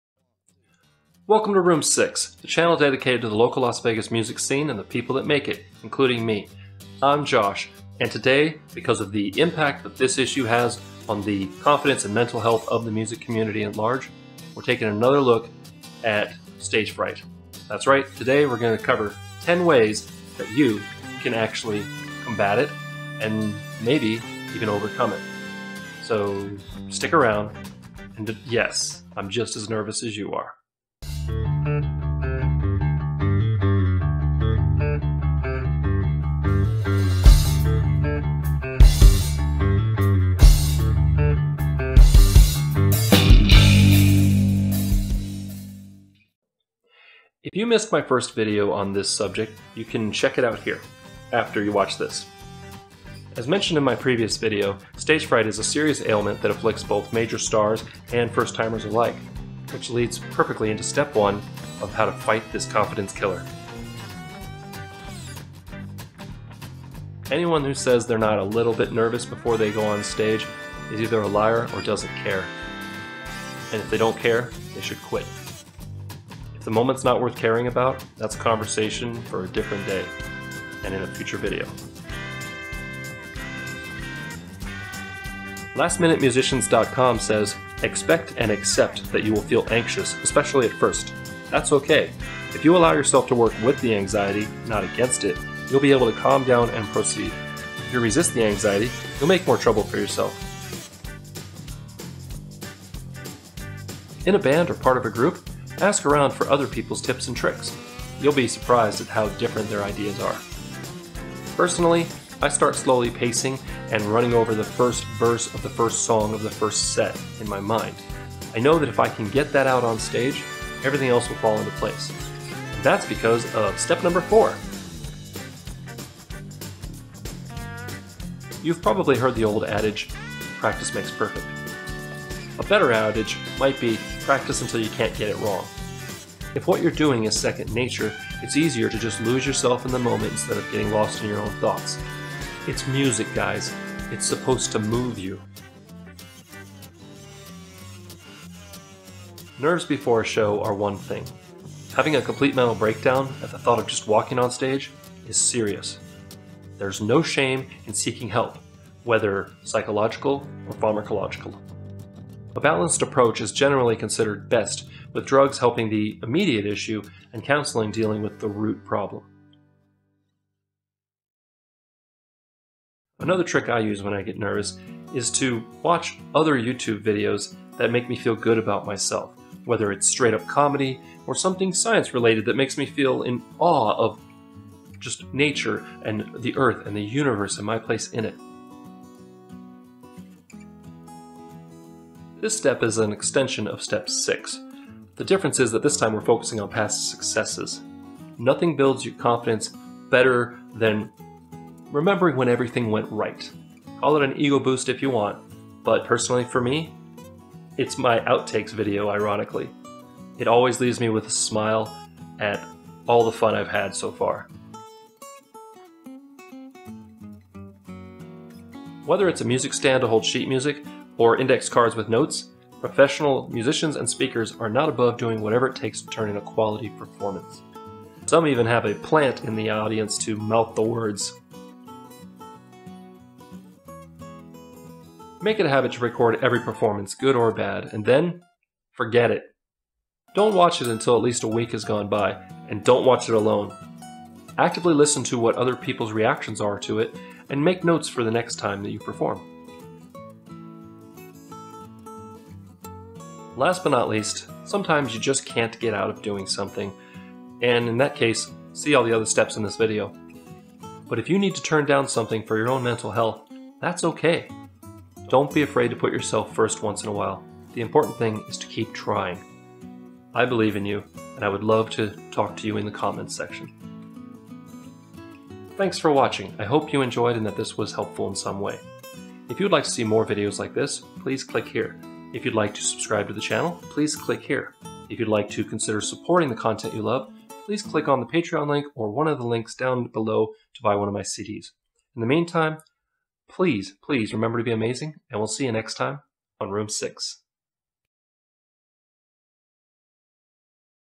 ROOM 6 SOUND BITES #21 - 10 More Ways to Fight Stage Fright! [TIPS/ADVICE]
Stage fright is still a very real problem for many musicians and public speakers. In this video, I share 10 more ways to face this issue and hopefully overcome it.